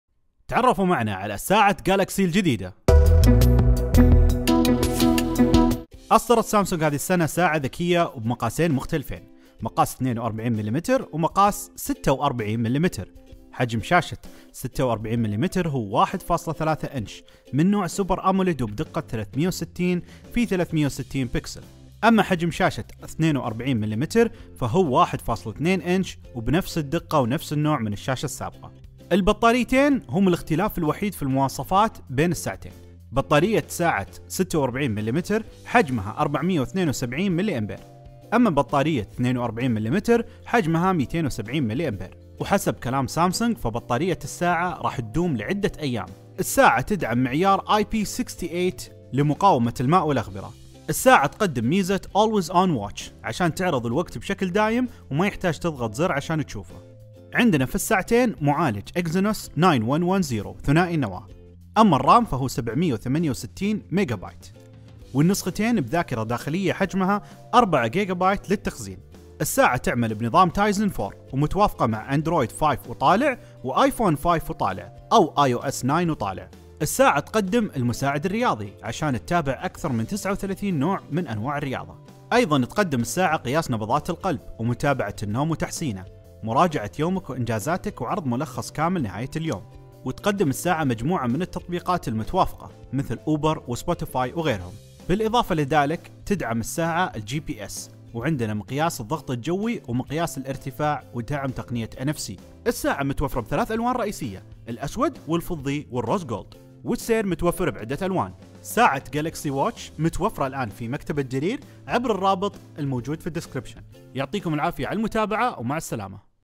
نظرة على ساعة Galaxy Watch
تعرفوا معنا على أحدث إصدارات سامسونج بالساعات الذكية، ساعة جالكسي واتش

ايش اكثر موديل اعجبكم منها؟

اضغط على الرابط للشراء من الموقع Click to purchase online

Twitter تويتر

Instagram انستغرام

Snap Chat سناب تشات